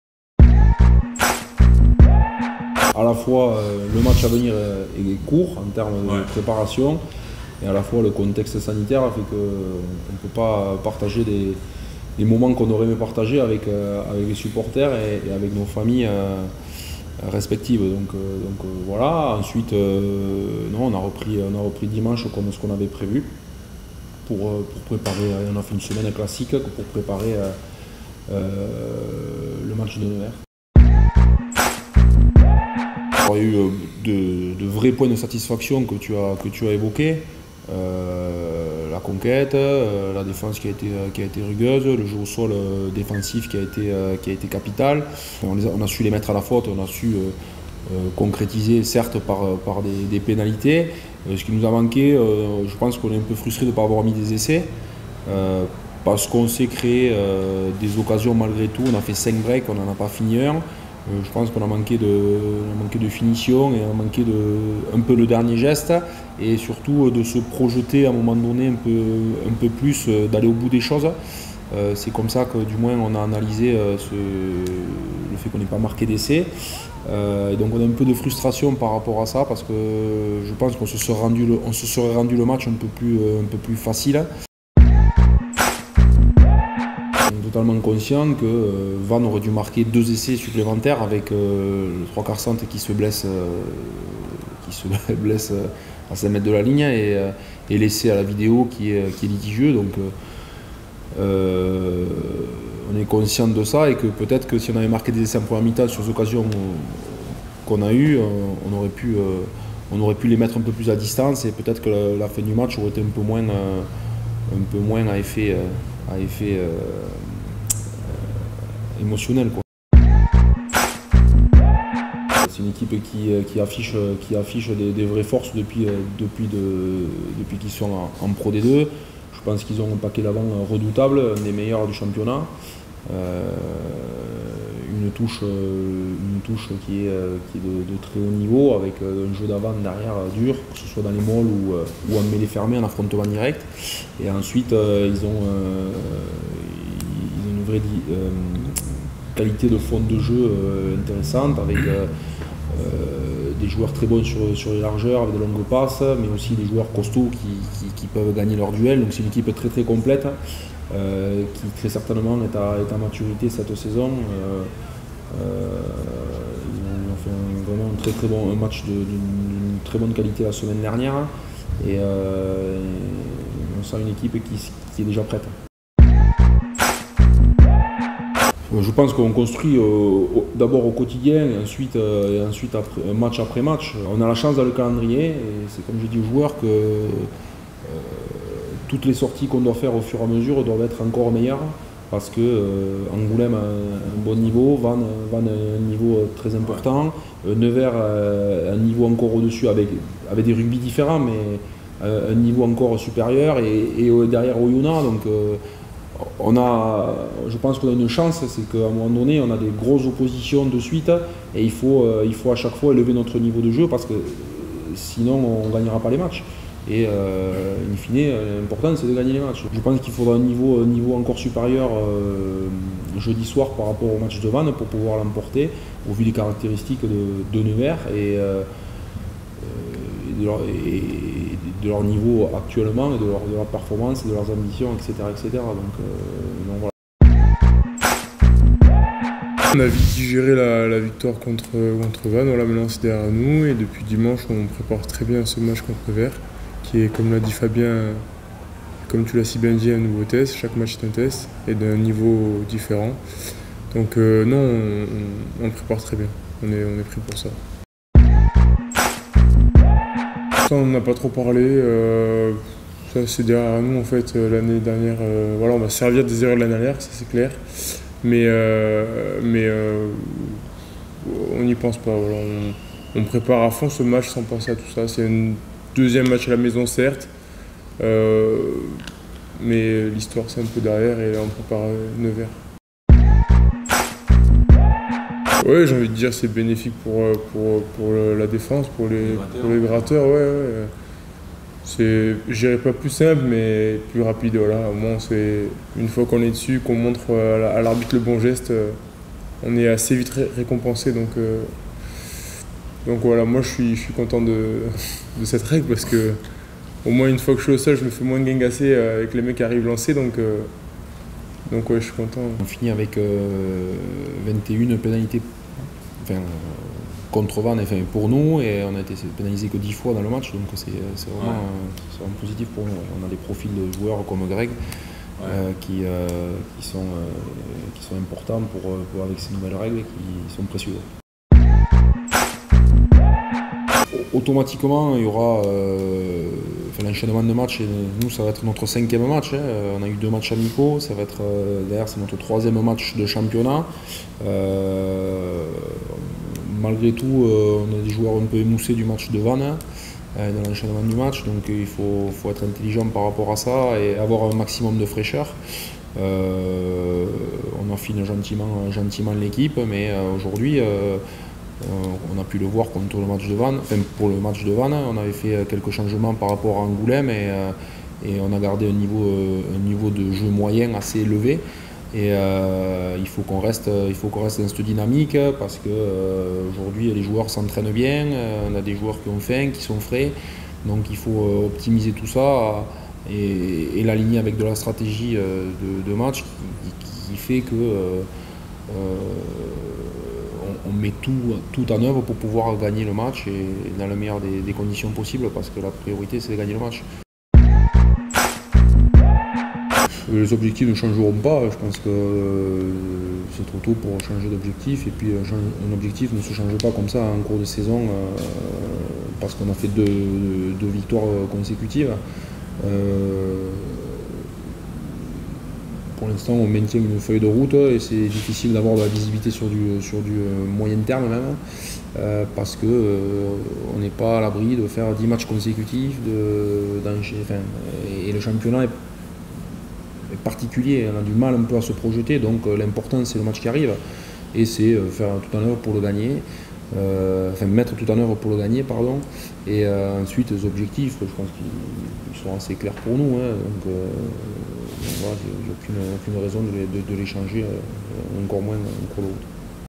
Point presse avant Provence/Nevers (15/09/2020)
Avant la réception de Nevers au stade Maurice-David, ce jeudi à 20h45, Fabien Cibray et Grégory Annetta répondaient aux questions des journalistes.